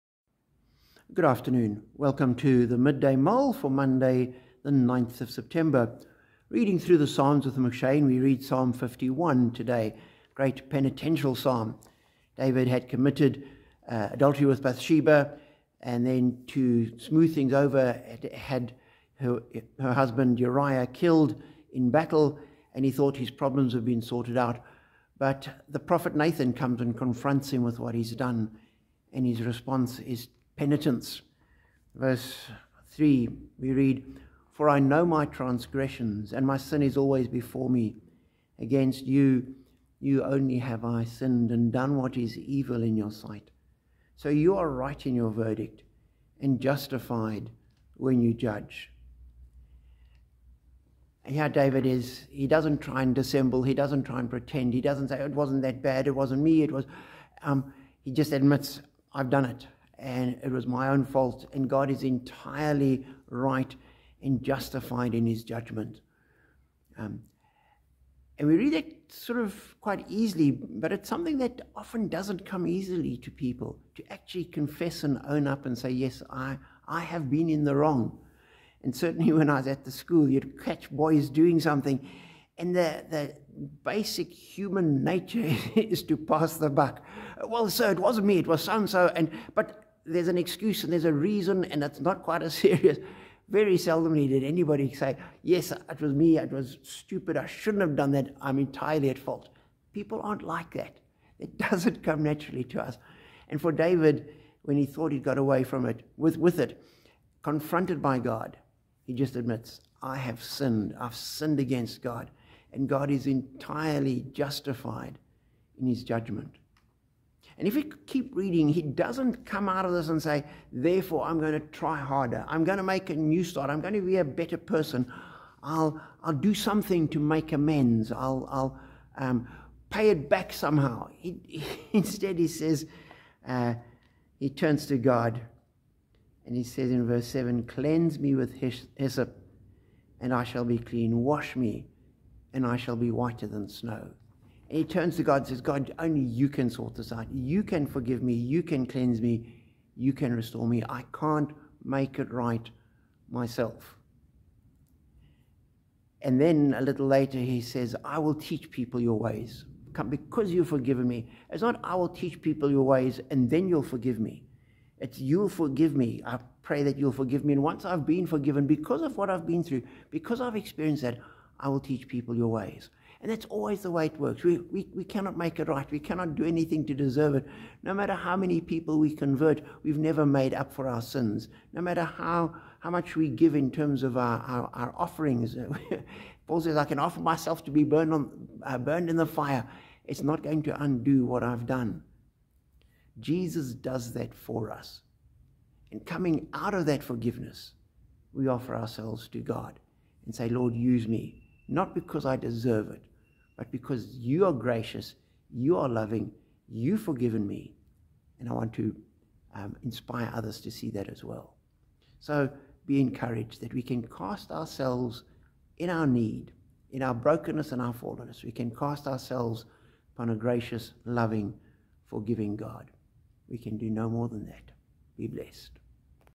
Midday Mull 24 09 09
When we fail, which we do all too often, all we can do is acknowledge our failure and cast ourselves on God.  (Psalm 51:3-7)